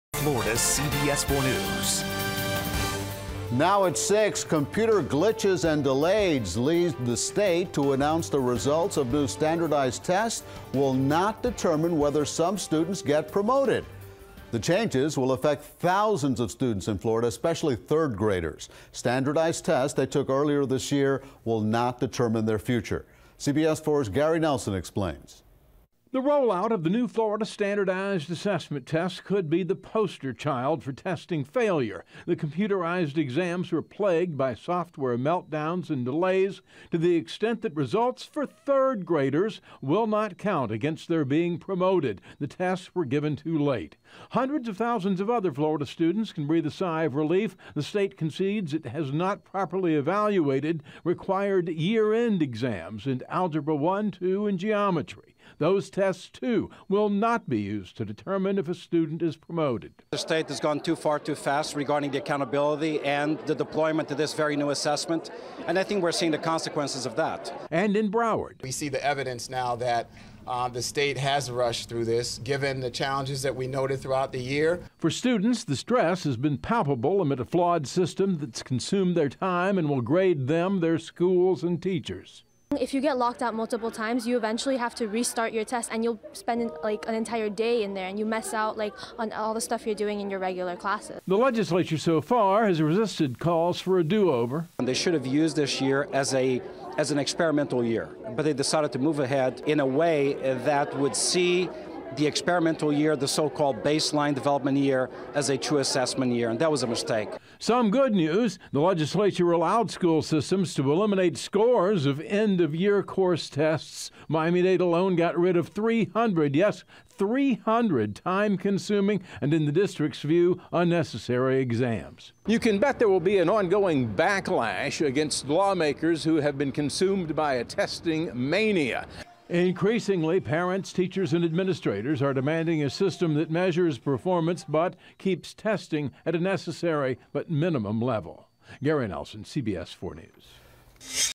Test Grade Eliminations Result Of State Going "Too Far, Too Fast"
Miami-Dade Public Schools Superintendent Alberto Carvalho spoke out Tuesday after Monday’s big news when it comes to testing students in South Florida.